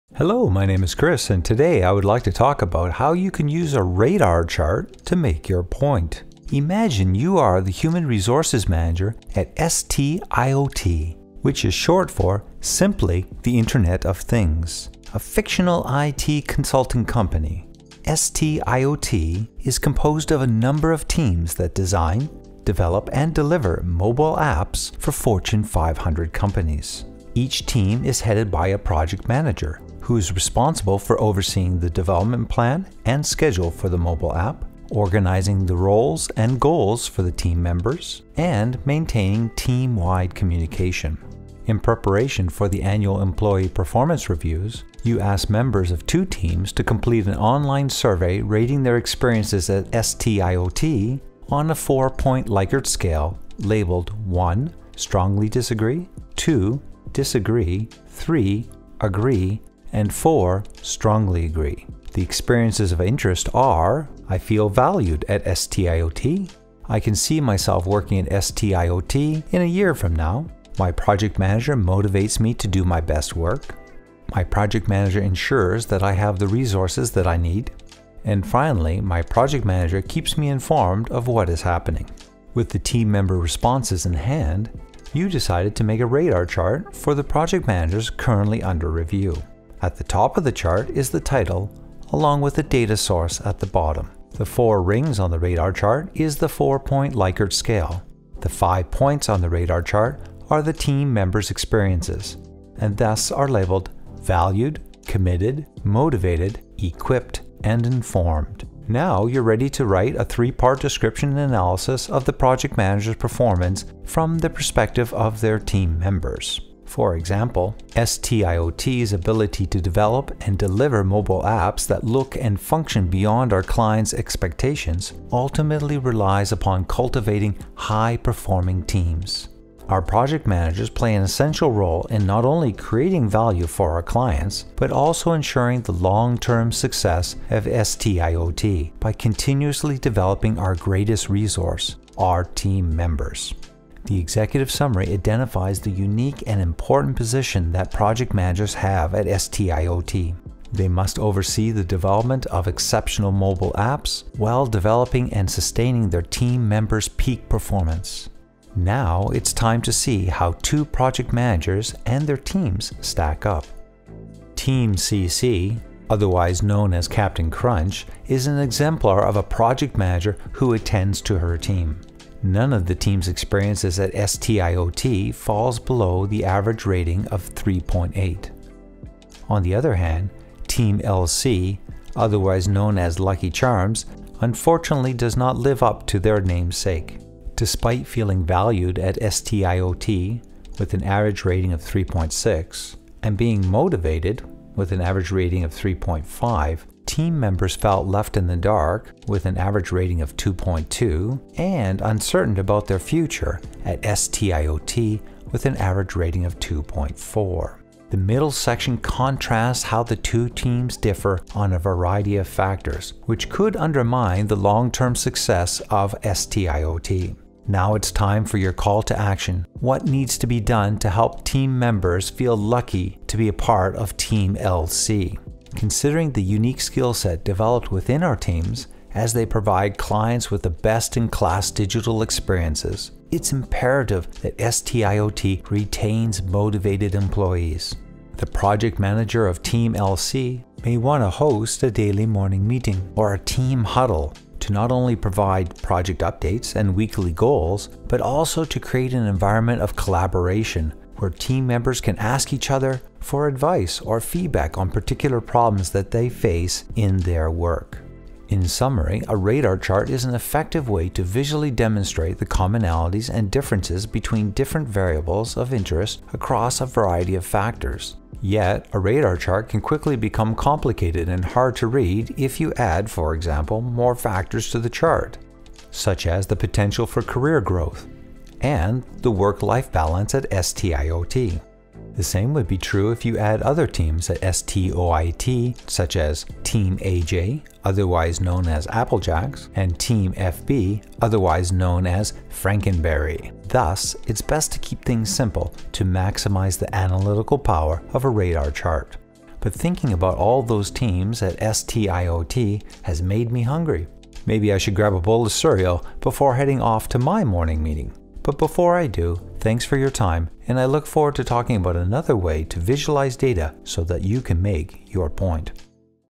Use a Radar Chart to Explain Your Data
Today will we talk about how to use a radar chart to compare the performance of two project managers on multiple factors. Don't worry. It can all be done with a three-part description and analysis.  Here is an example for a fictional IT consulting company.

“STIOT’s ability to develop and deliver mobile apps that look and function beyond our client’s expectations ultimately relies upon cultivating high-performing teams. Our project managers play an essential role not only creating value for our clients, but also ensuring the long-term success of STIOT by continuously developing our greatest resource – our team members. Team C.C. (a.k.a. Captain Crunch) is an exemplar of a team with a project manager who attends to her team. None of the team’s experiences at STIOT falls below an average rating of 3.8. On the other hand, Team L.C. (a.k.a. Lucky Charms) unfortunately does not live up to their namesake. Despite feeling valued at STIOT (3.6) and motivated (3.5), team members felt left in the dark (2.2) and uncertain about their future at STIOT (2.4). Considering the unique skill set developed within our teams as they provide clients with best-in-class digital experiences, it is imperative that STIOT retains motivated employees. The Team L.C. Project Manager may want to host a daily morning meeting or a “team huddle” to not only provide project updates and weekly goals, but also to create an environment of collaboration where team members can ask others for advice or feedback on particular problems they face in their work.”